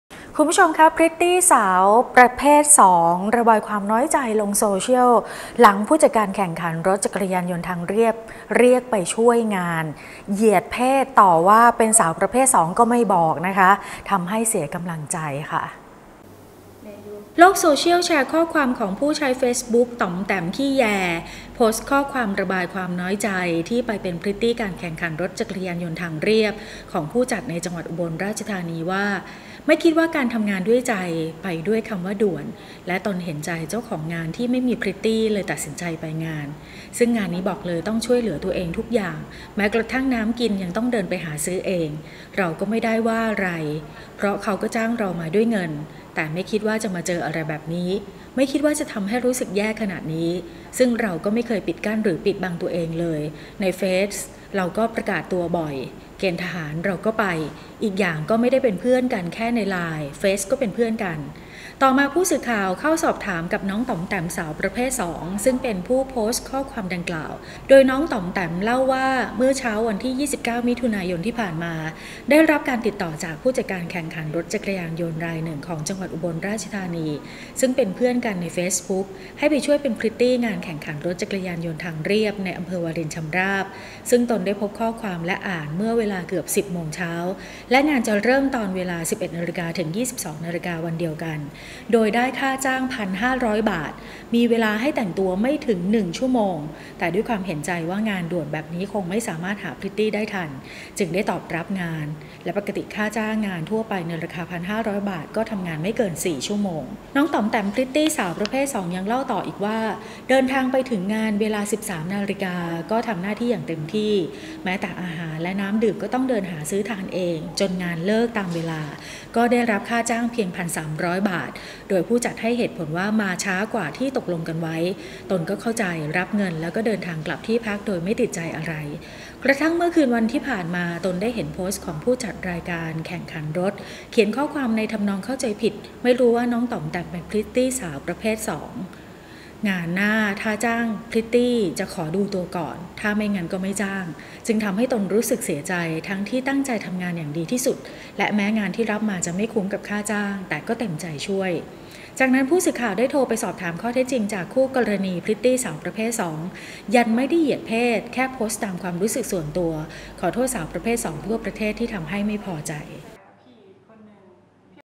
พริตตี้สาว 2 โพสต์น้อยใจโดนเหยียดเพศ
พริตตี้สาวประเภท 2 ระบายความน้อยใจลงโซเชียล หลังผู้จัดการแข่งขันรถจักรยานยนต์ทางเรียบ เรียกไปช่วยงาน เหยียดเพศต่อว่าเป็นสาวประเภทก็ไม่บอก ทำให้เสียกำลังใจ